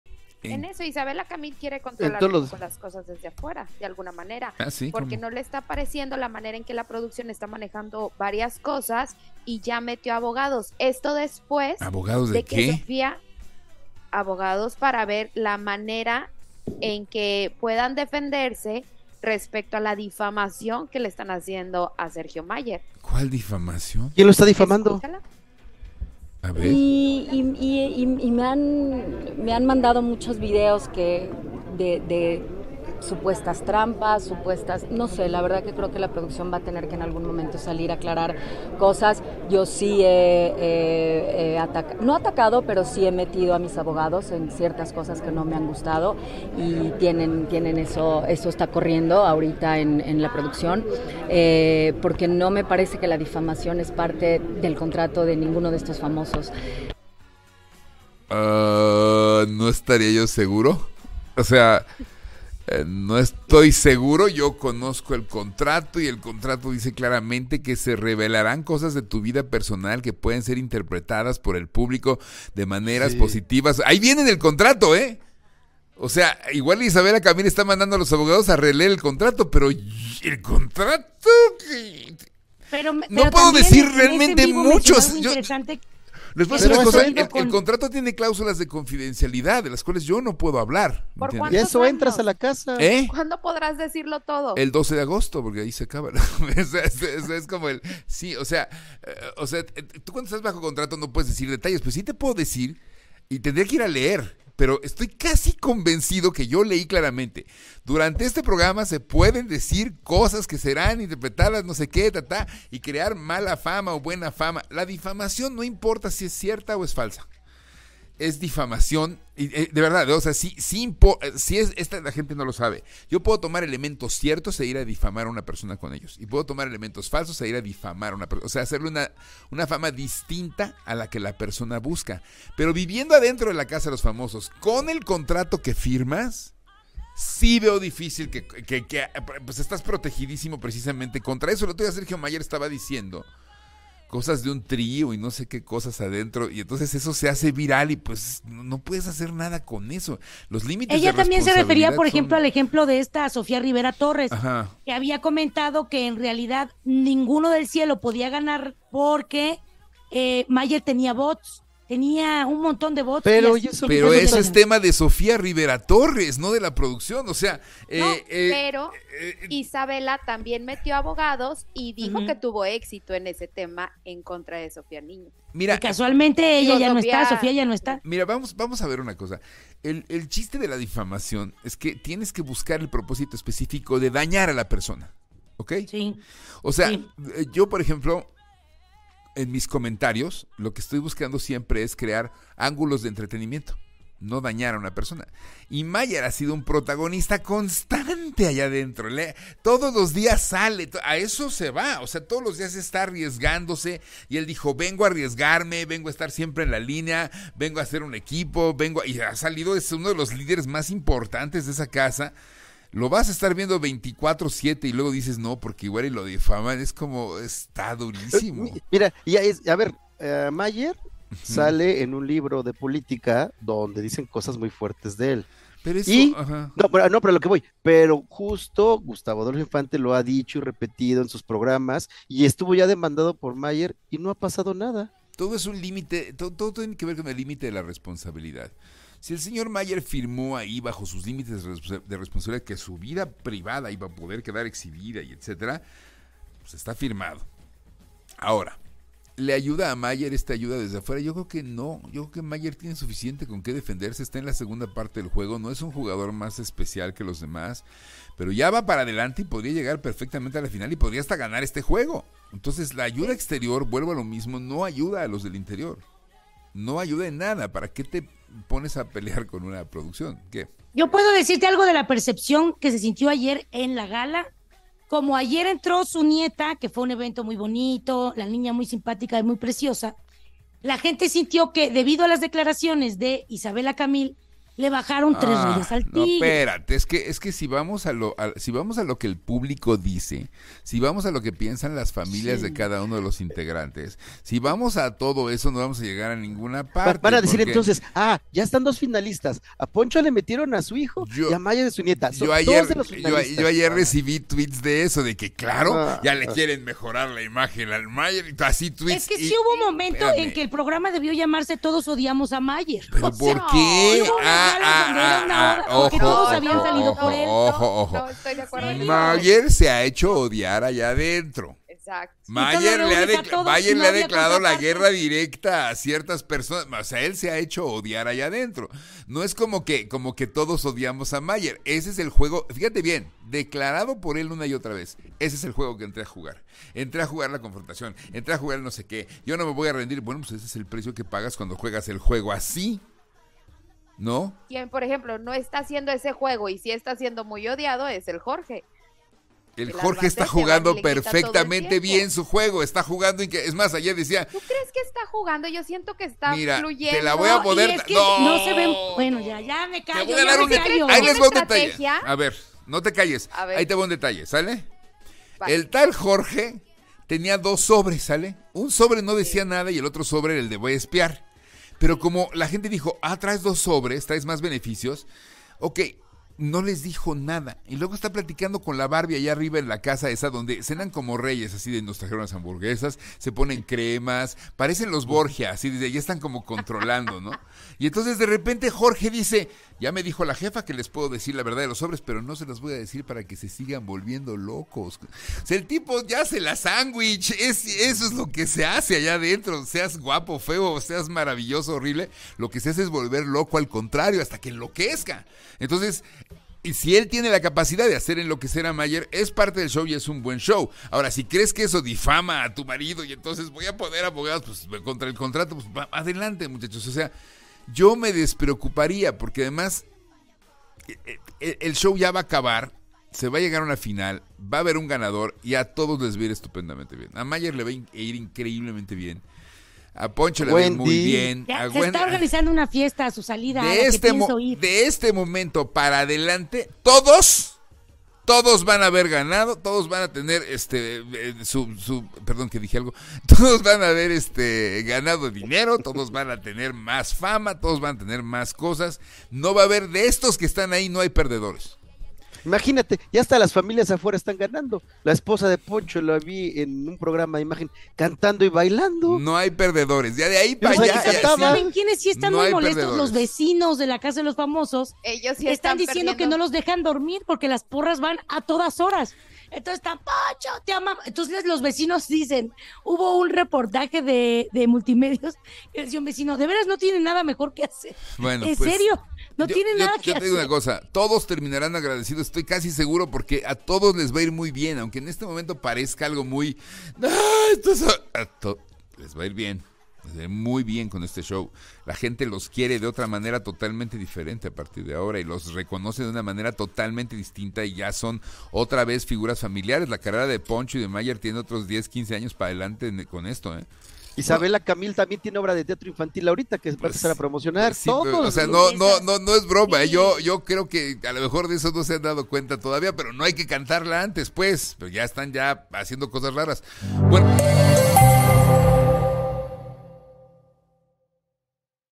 Issabela Camil acusa a LCDLF MX de difamar a Sergio Mayer
Issabela Camil asegura que la producción de ‘La Casa de los Famosos México’ (LCDLF MX), ha estado realizando una difamación en contra del expolítico Sergio Mayer, menciona que ella ha estado mandando a sus abogados para conseguir respuestas y que tarde o temprano la producción tendrá que salir a dar respuestas de estas supuestas difamaciones.
Grupo Fórmula

Canal sugerido

Descarga nuestra